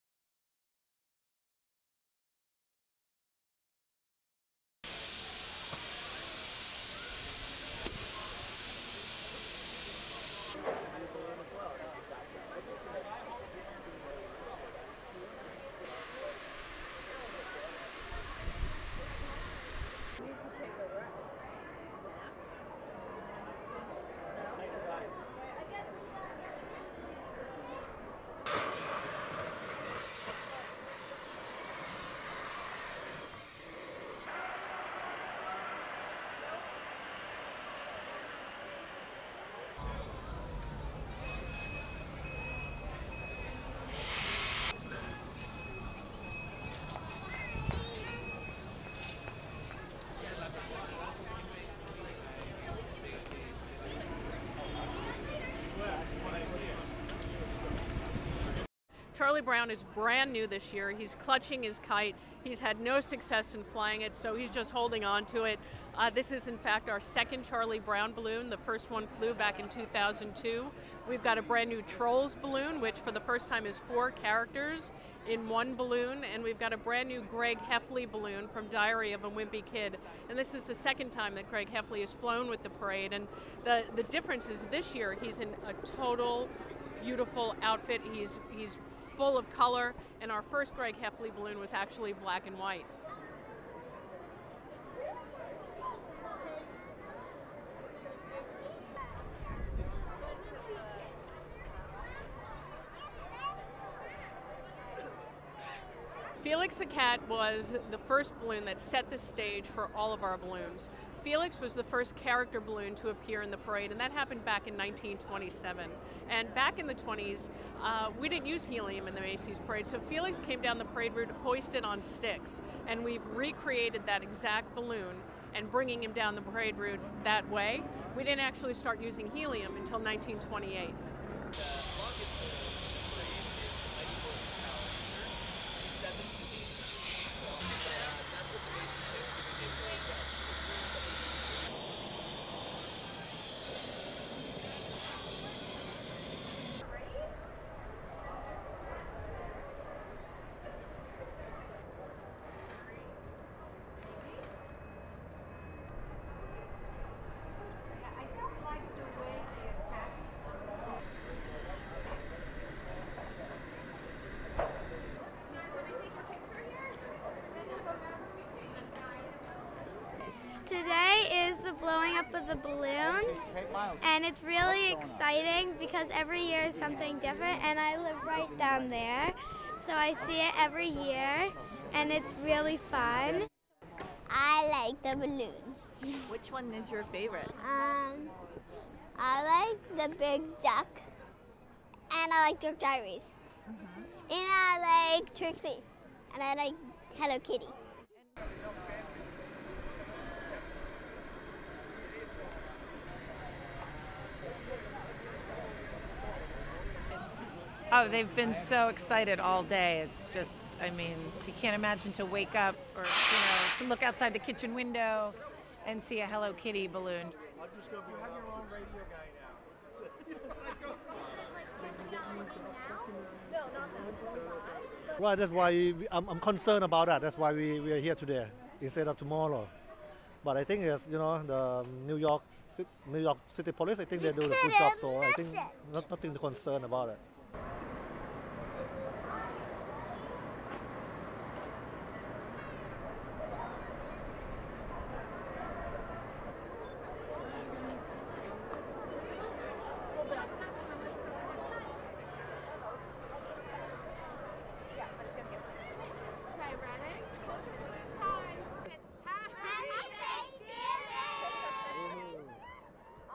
Giants balloons are inflated for Macy's Thanksgiving Day Parade
The 90th Macy's Thanksgiving Day Parade has a line-up of famous characters, with newcomers including a color version Greg Heffley from the Diary of a Wimpy Kid, a kiting Charlie Brown and a throwback to the past with Felix the Cat that is modeled after the 1927 original.